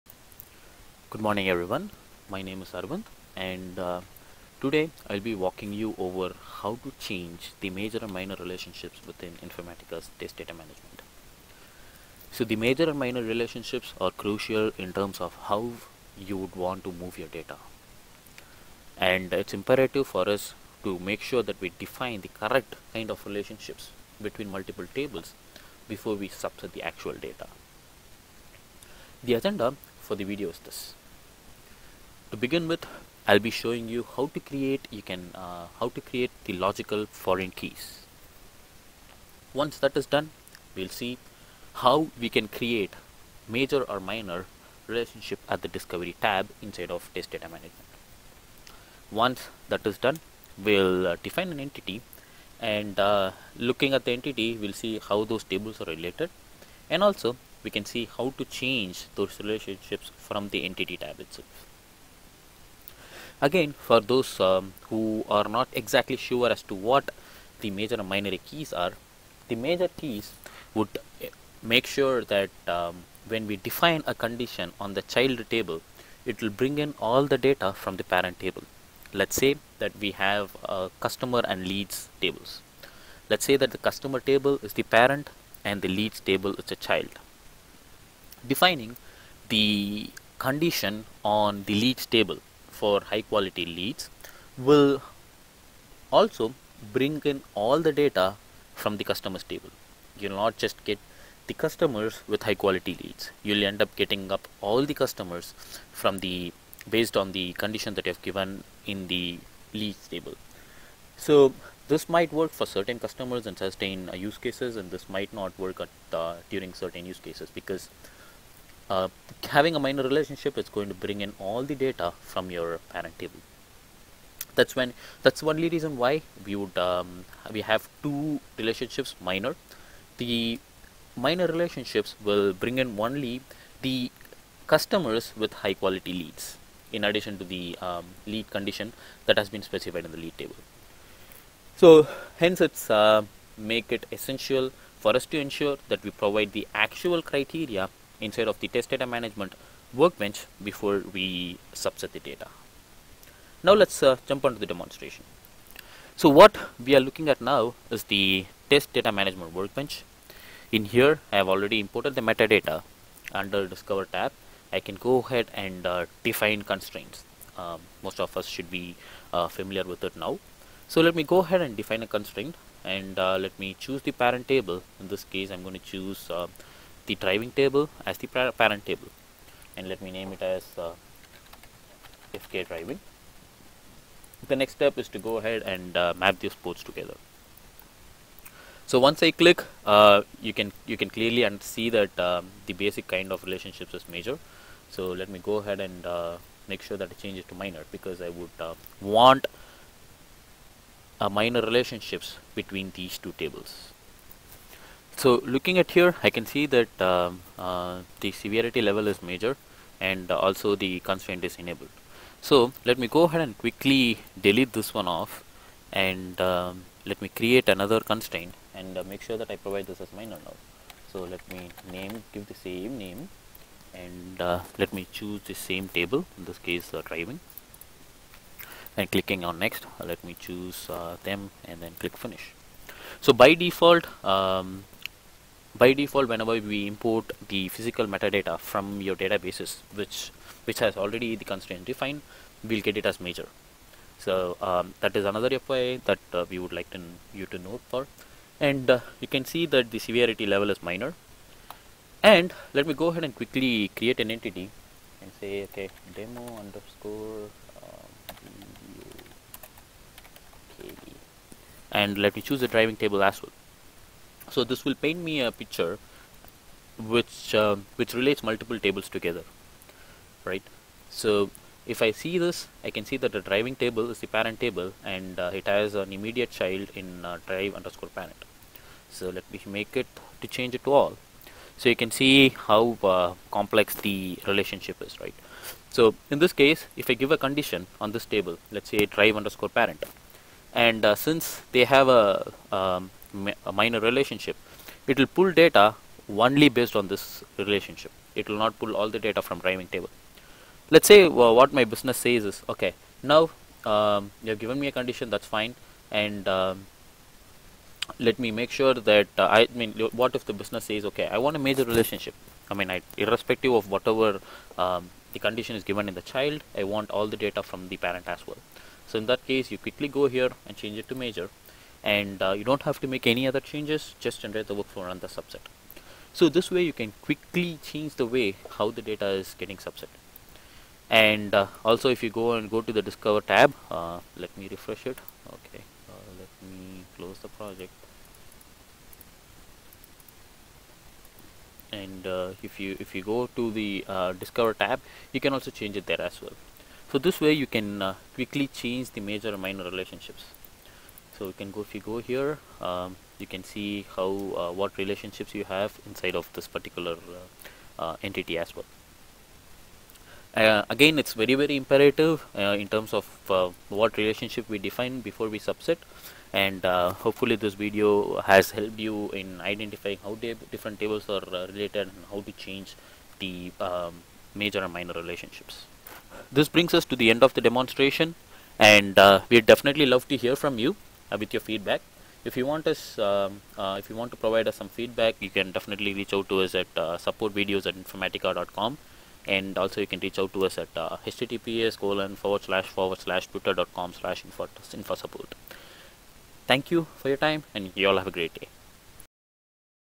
How to change Major-Minor Relationships in Test Data Management 9.5.2 HotFix 3
This video describes how to change the Major-Minor relationships in Test Data Management 9.5.2 Hotfix 3